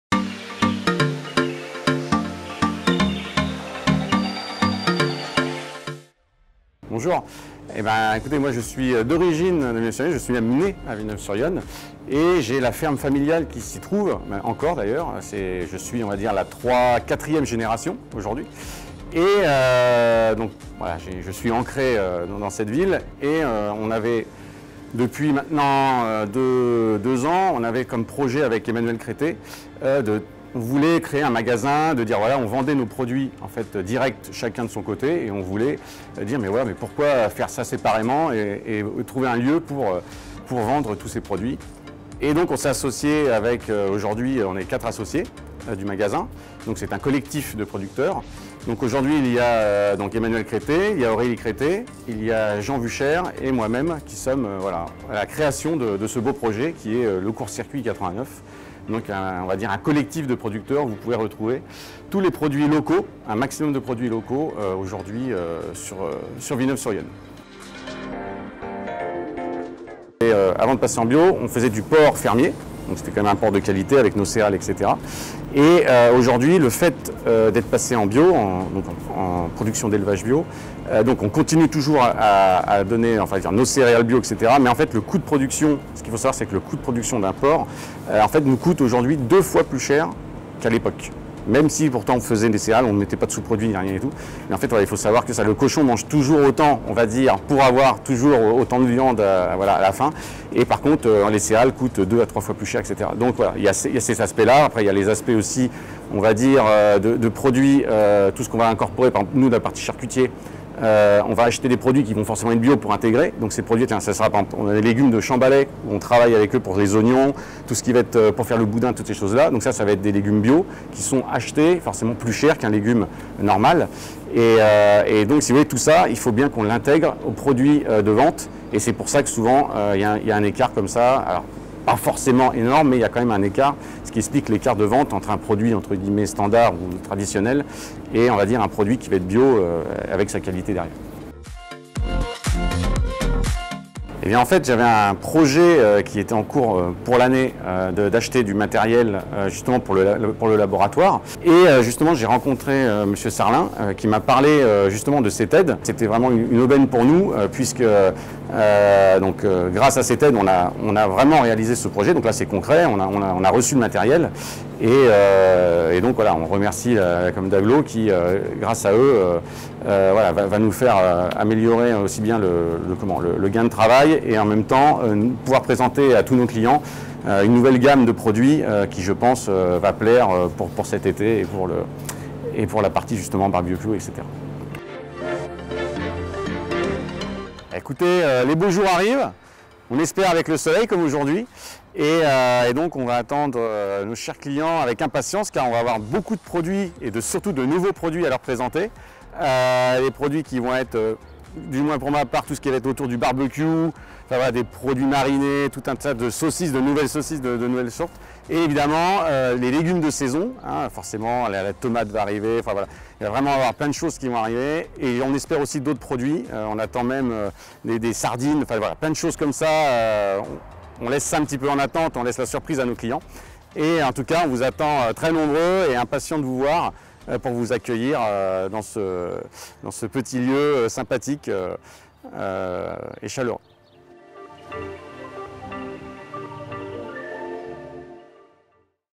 Le Court-circuit, Villeneuve-sur-Yonne
Comment le fonds régional des territoires a permis à ces commerçants d'investir dans du matériel professionnel?

Le court-circuit
42 bis route de Beaudemont - 89500 Villeneuve-sur-Yonne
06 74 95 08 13